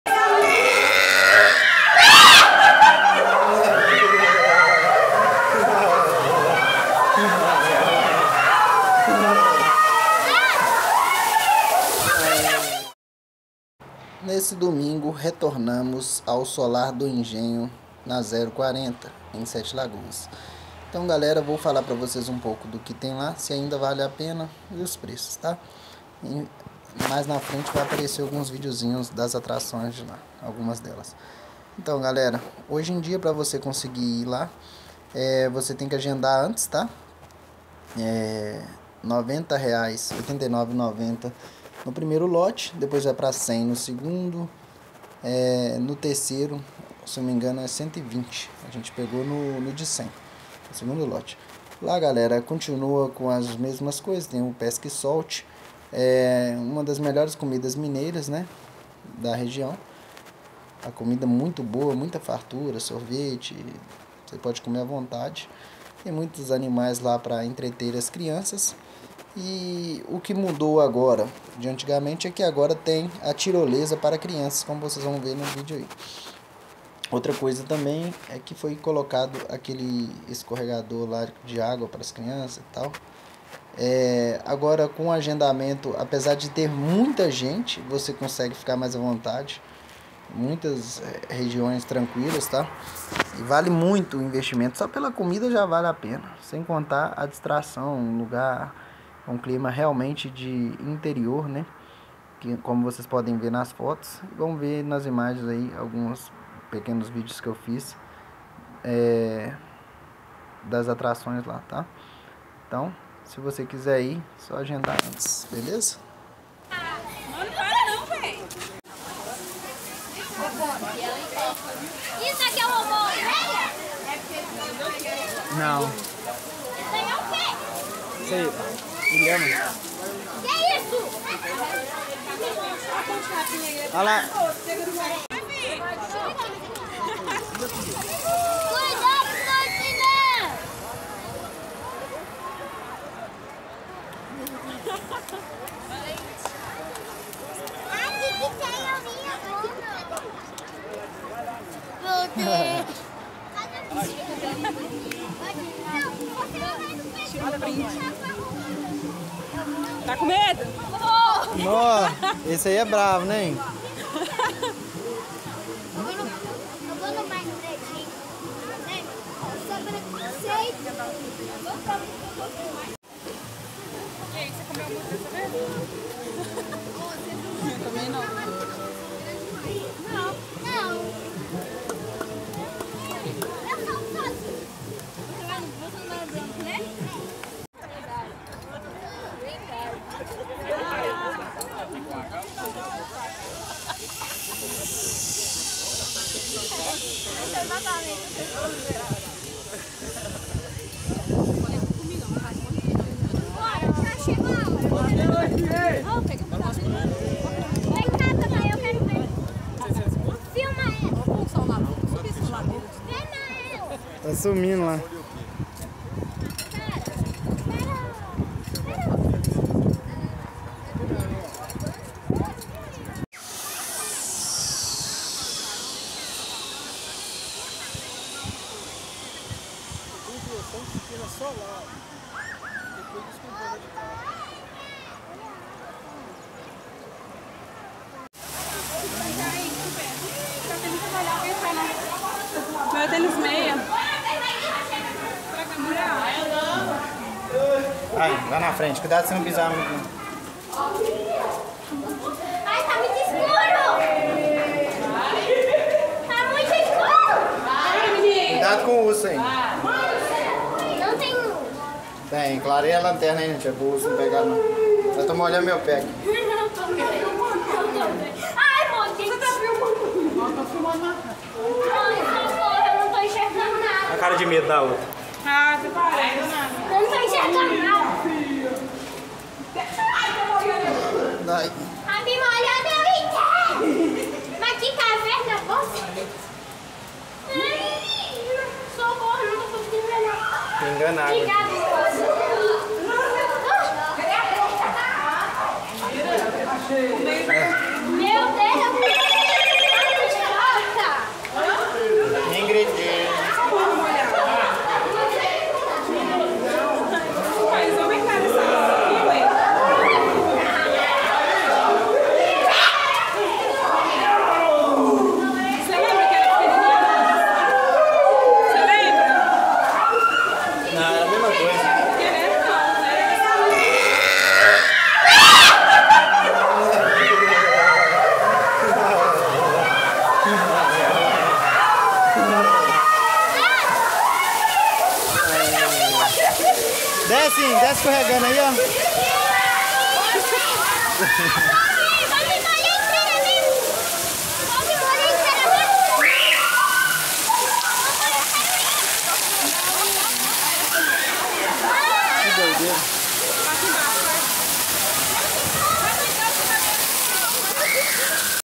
SOLAR DO ENGENHO 2023 PREÇO E ATRAÇÕES
Solar do engenho vale a pena?

Domingo é dia de passear então vem comigo pro engenho.